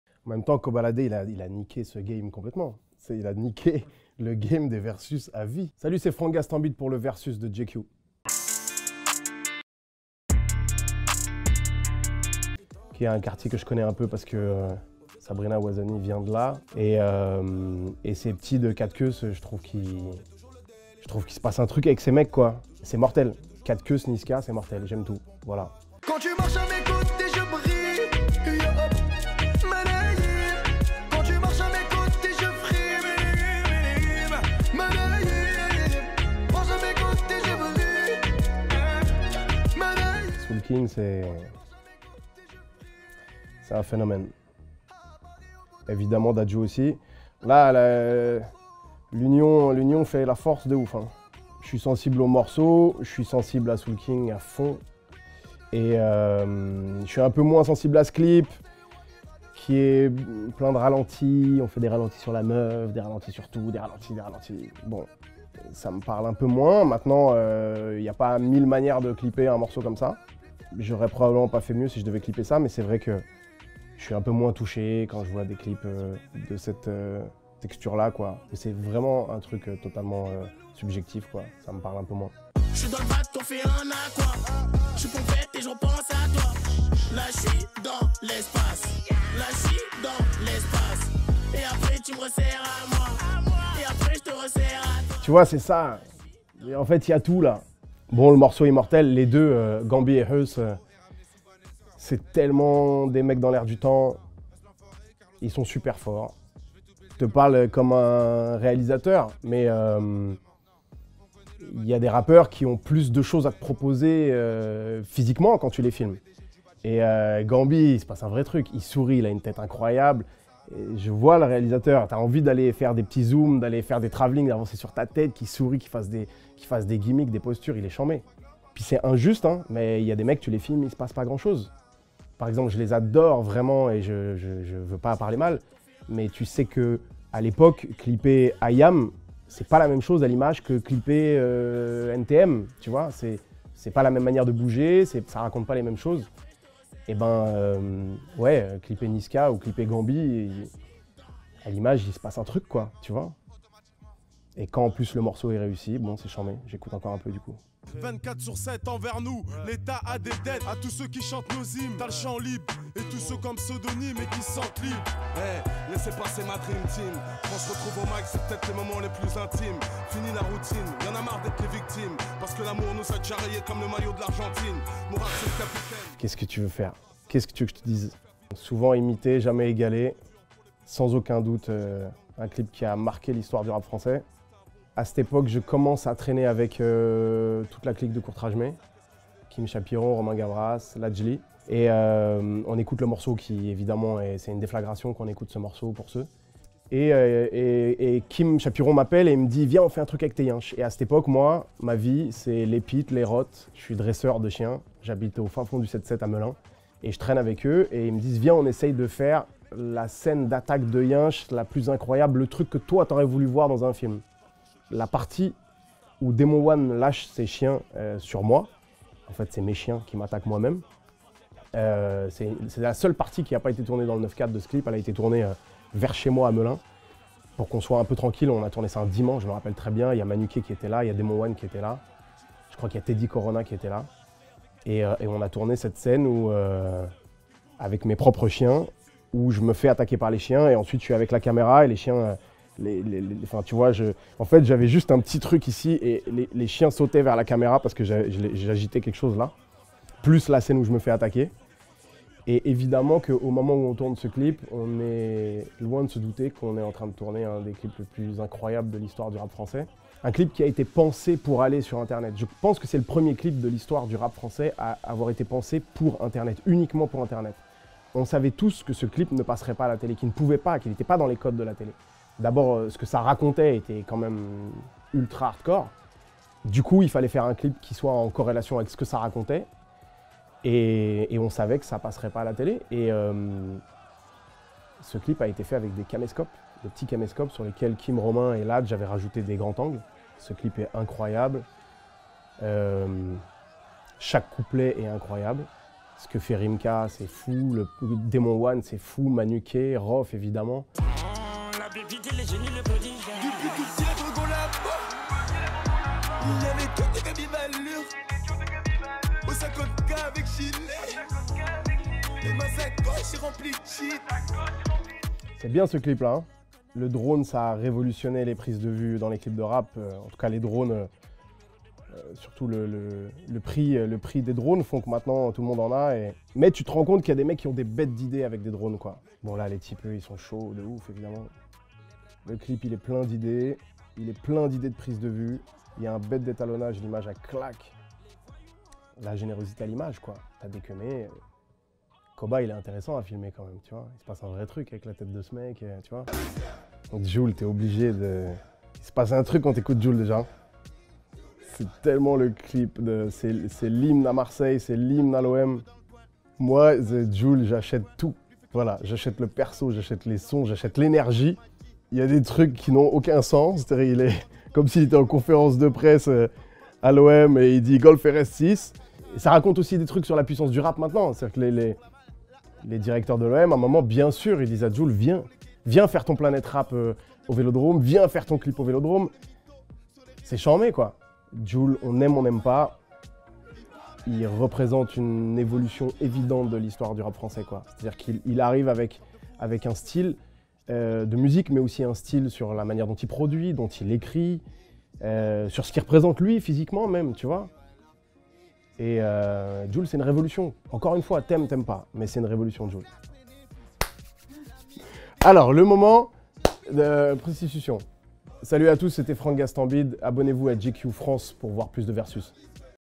Franck Gastambide juge le rap français : Koba LaD, Chily, Heuss L'enfoiré… | GQ
Alors que Validé, la première série française sur le rap qu’il a écrit, réalisé et dans laquelle il joue a cartonné sur Canal plus Franck Gastambide est passé à la rédaction de GQ France pour donner son avis sur le rap français, de la nouvelle génération aux personnalités historiques. Koba LaD, Jul, Heuss l’enfoiré... tous sont passés sous le radar de la star montante du cinéma français. En 2023, il joue dans le dernier "Astérix et Obélix : l'Empire du Milieu" de Guillaume Canet.